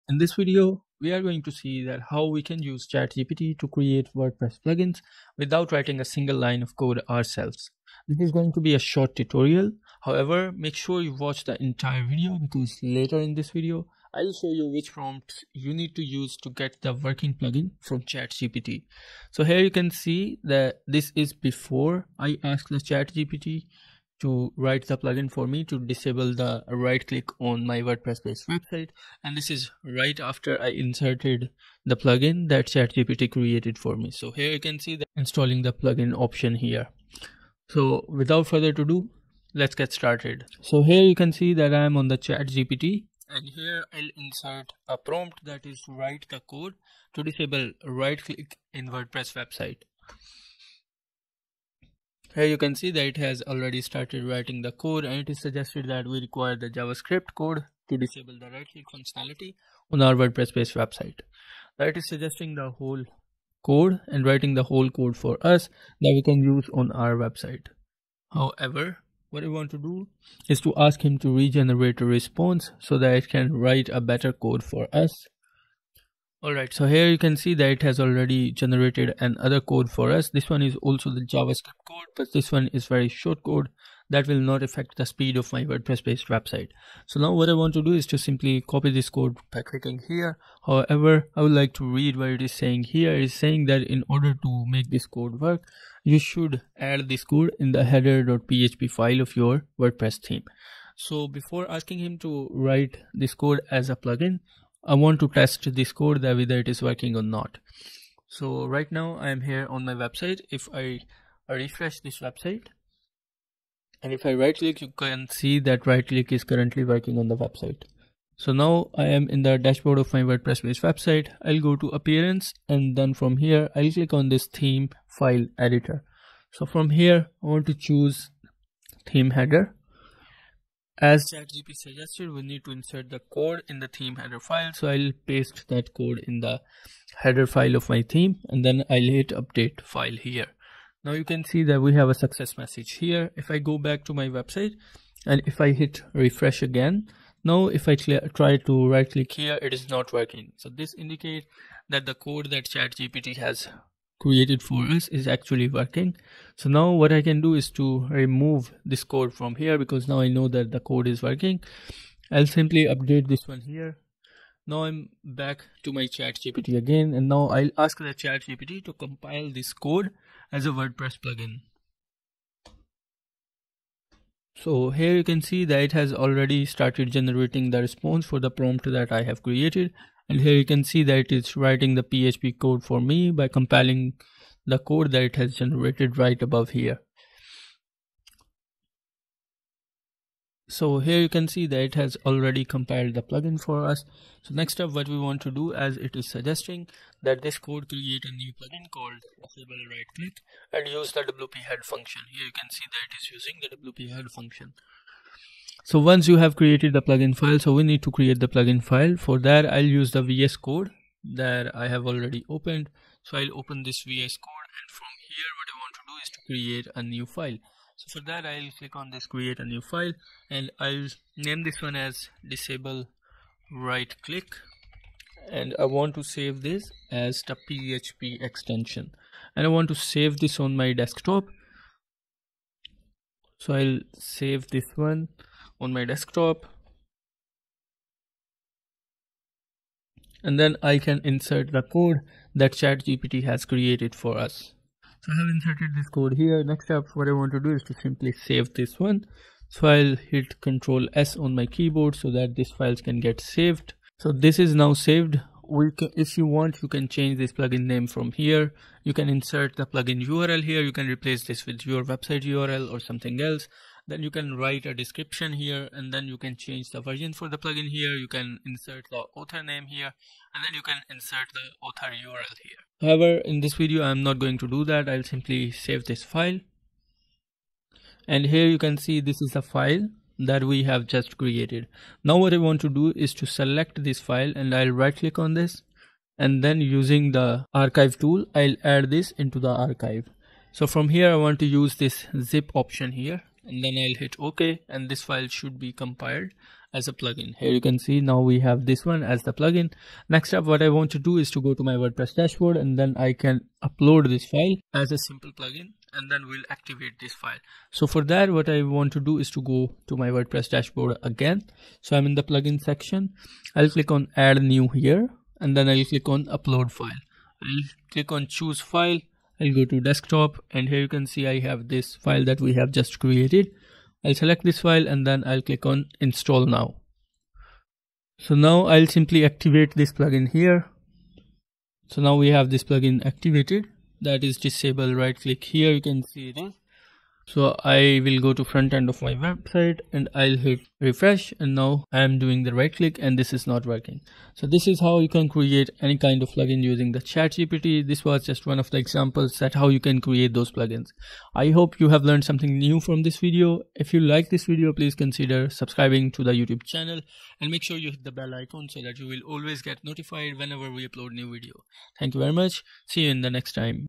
How To Use ChatGPT to Create Wordpress Plugin in 2023 - [EASY TUTORIAL]
In this video, we're exploring how to use AI to create a powerful Wordpress plugin in 2023. Using the cutting-edge technology of ChatGPT, we'll show you how to enhance your website's functionality and improve user experience.